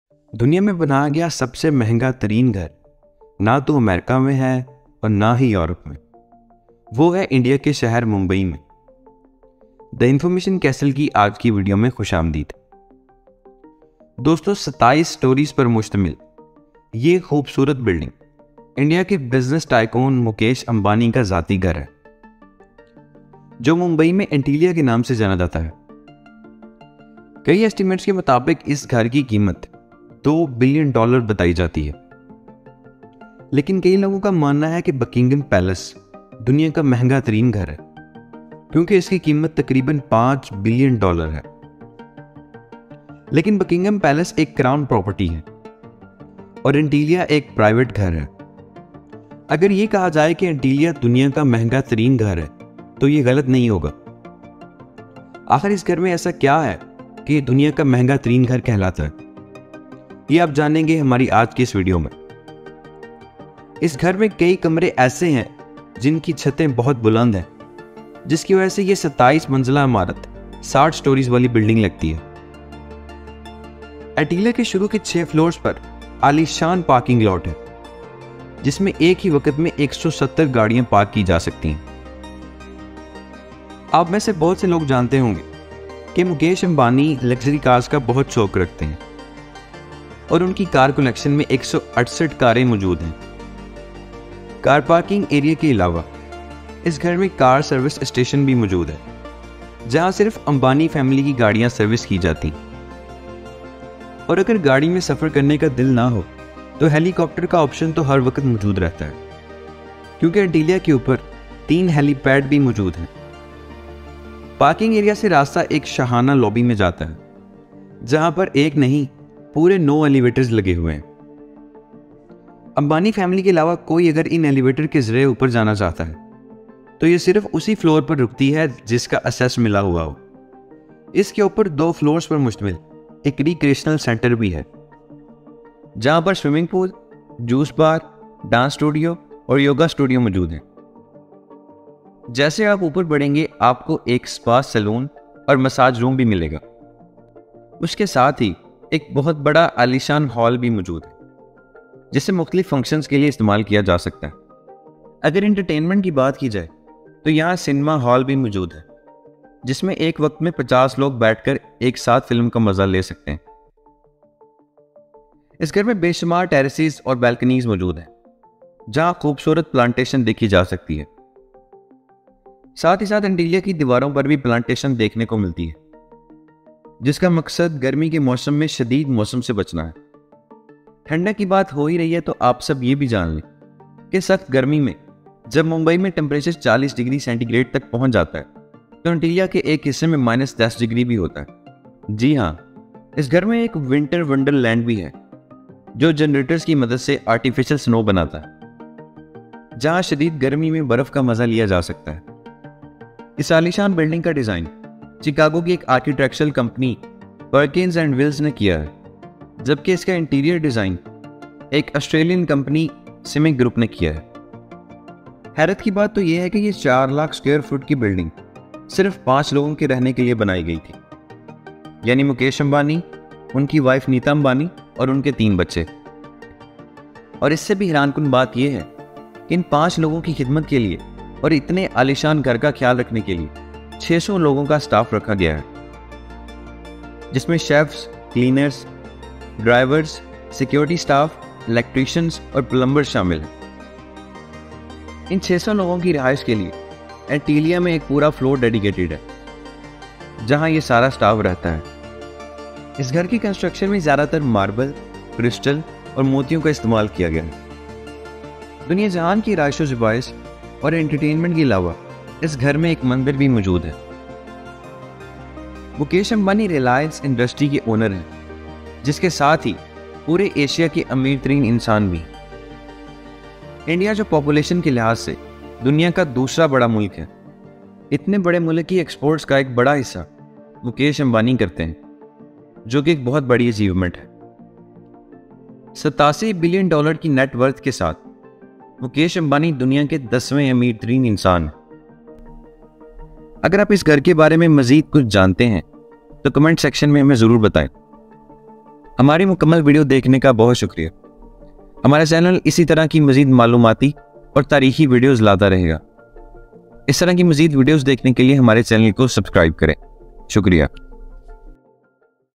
Most Expensive House In The World | Mukesh Ambani House | Antilia
Antilia is one of the most expensive private residences in the world, located in Mumbai, India. This architectural marvel is owned by Mukesh Ambani, one of the richest men in Asia. With 27 floors and over 400,000 square feet of living space, Antilia is a true symbol of luxury and extravagance. This video takes you on a virtual tour of Antilia and explores the incredible features and amenities that make this residential skyscraper a true wonder of modern engineering and design.